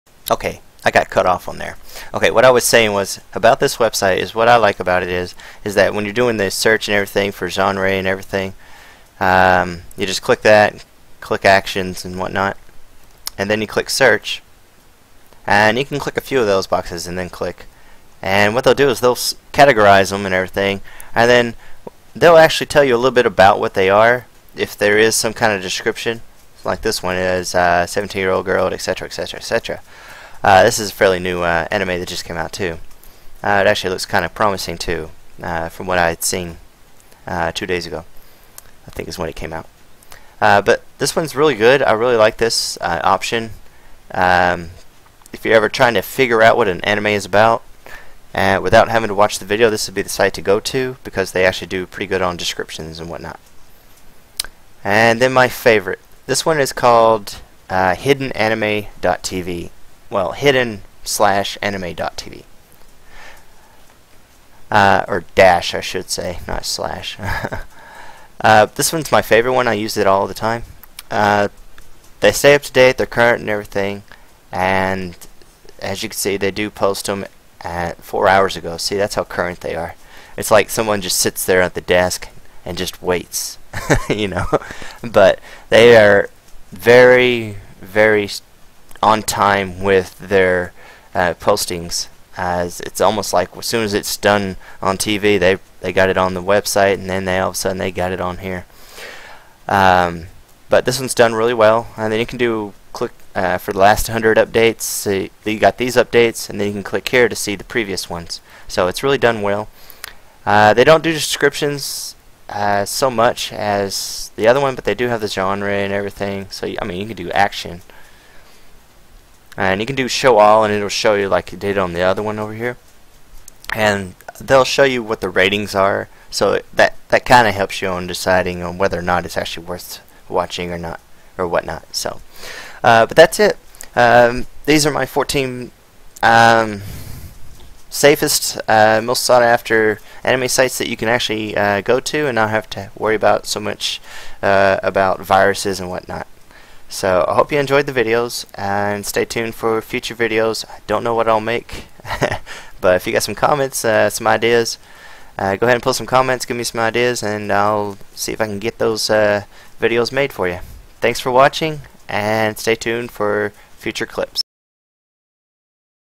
2012 Best Anime Sites - to watch anime- part 2 of 2
Listed are 14 websites safe for use by anime fans-  This is a two part video of websites you can watch your favorite and newly posted anime on .